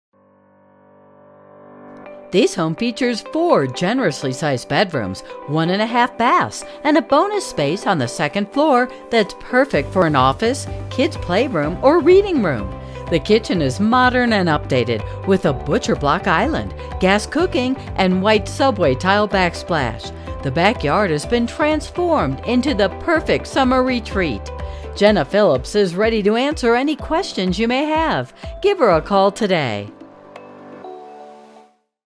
314 E New Street Lancaster, PA | ColdwellBankerHomes.com
314 E New Street, Lancaster, PA

Welcome home to E New Street where youll find all the character youd expect in a city home perfectly paired with modern updates. This home features four generously sized bedrooms, one and one half bath, and a bonus space on the second floor that is perfect for an office, kids playroom, or reading